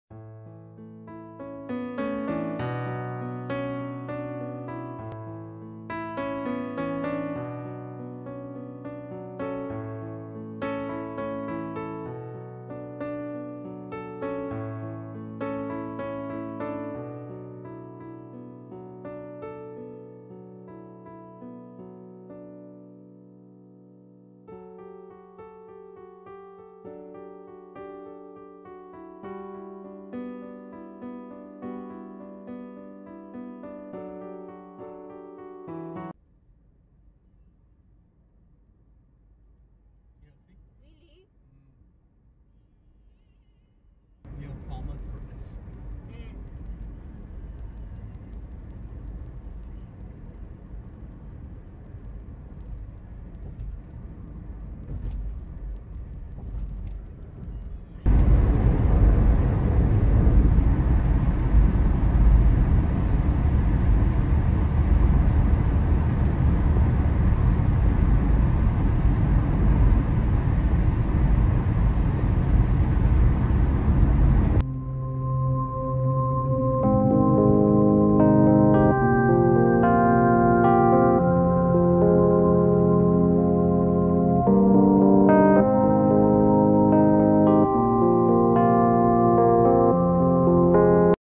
You feel better when you watch the sunrise/Sunrise is good for the health
You feel better when you watch the sun rises and make your mood better when you wake up with the sunrise.